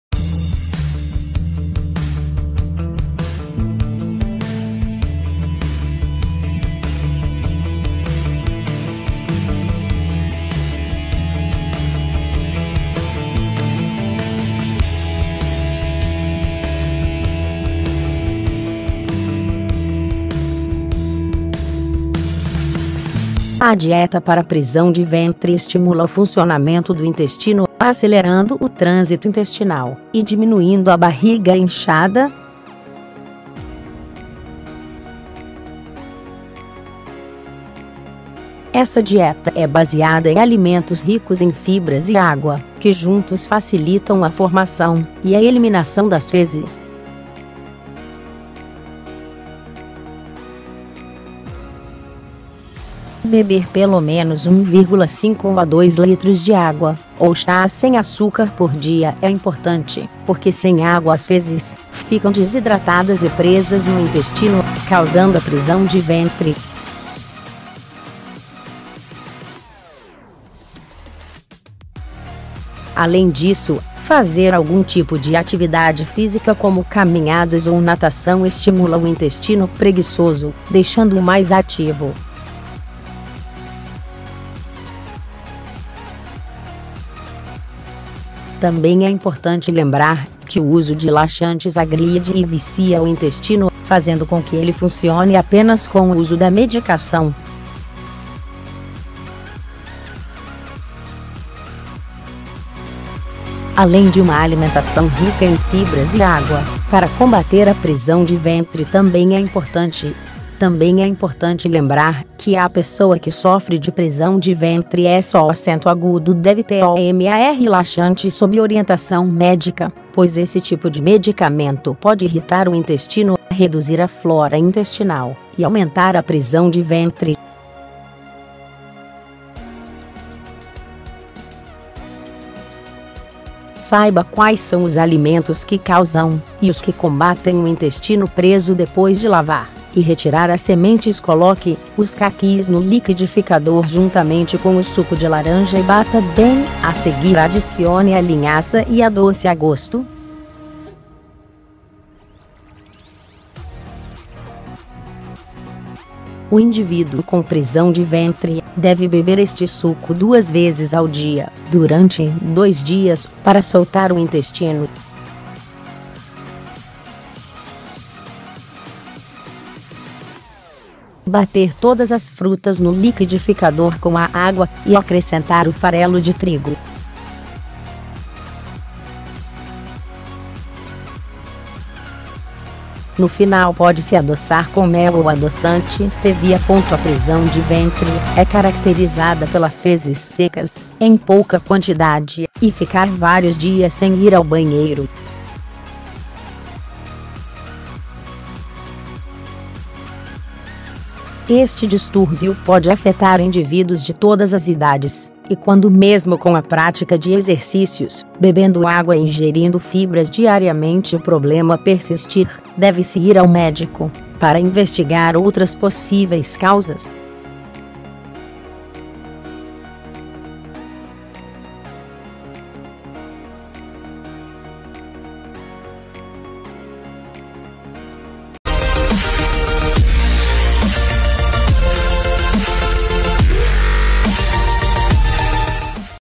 Saúde - Dieta para prisão de ventre
Adieta para prisão de ventre estimula o funcionamento do intestino, acelerando o trânsito intestinal e diminuindo a barriga inchada.Essa dieta é baseada em alimentos ricos em fibras e água, que juntos facilitam a formação e a eliminação das fezes.Beber pelo menos 1,5 a 2 litros de água ou chá sem açúcar por dia é importante porque sem água as fezes ficam desidratadas e presas no intestino, causando a prisão de ventre.Além disso, fazer algum tipo de atividade física como caminhadas ou natação estimula o intestino "preguiçoso", deixando-o mais ativo.Também é importante lembrar que o uso de laxantes agride e vicia o intestino, fazendo com que ele funcione apenas com o uso da medicação.Além de uma alimentação rica em fibras e água, para combater a prisão de ventre também é importante:  Também é importante lembrar que a pessoa que sofre de prisão de ventre só deve tomar laxantes sob orientação médica, pois esse tipo de medicamento pode irritar o intestino, reduzir a flora intestinal e aumentar a prisão de ventre.Saiba quais são os alimentos que causam e os que combatem o intestino preso  Depois de lavar e retirar as sementes coloque os caquis no liquidificador juntamente com o suco de laranja e bata bem, a seguir adicione a linhaça e adoce a gosto.O indivíduo com prisão de ventre deve beber este suco 2 vezes ao dia, durante 2 dias, para soltar o intestino.Bater todas as frutas no liquidificador com a água e acrescentar o farelo de trigo.No final pode-se adoçar com mel ou adoçante stevia.A prisão de ventre é caracterizada pelas fezes secas, em pouca quantidade, e ficar vários dias sem ir ao banheiro.Este distúrbio pode afetar indivíduos de todas as idades, e quando mesmo com a prática de exercícios, bebendo água e ingerindo fibras diariamente o problema persistir, deve-se ir ao médico para investigar outras possíveis causas.